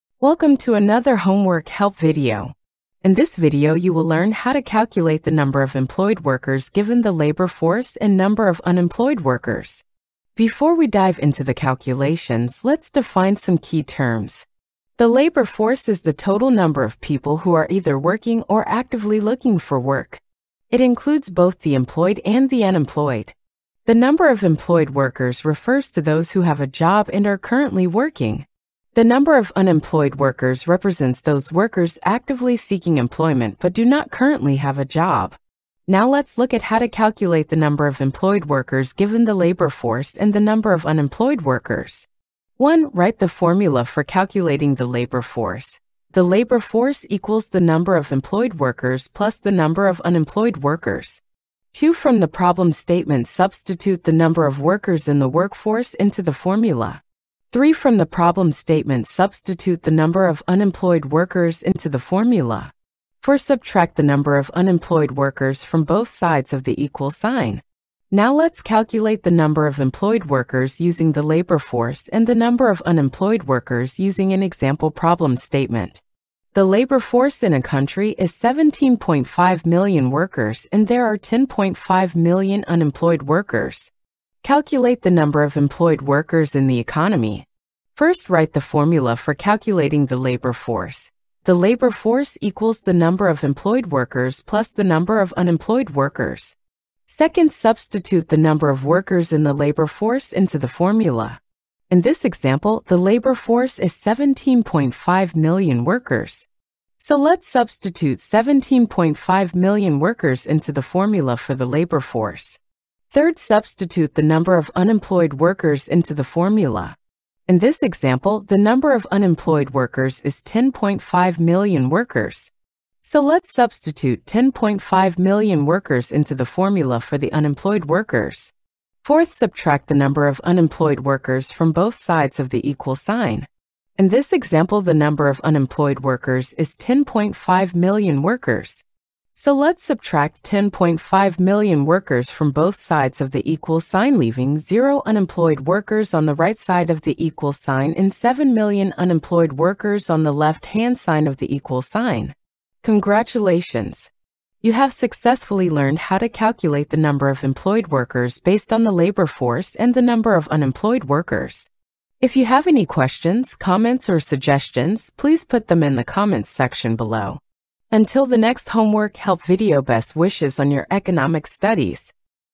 Calculating the Number of Employed Workers Given Labor Force and the number of Unemployed Workers
The video explains how to find the number of employed workers given the labor force and the number of unemployed workers in an economy
Narration Voice by TechSmith Audiate
Background by DALL-E
00:00 Introduction
00:14 Background
00:34 Steps for Solving
01:08 Example Problem Statement
01:27 Worked Example
02:32 Summary
If you would like to help cover expenses of producing this video, please buy me cup of coffee at: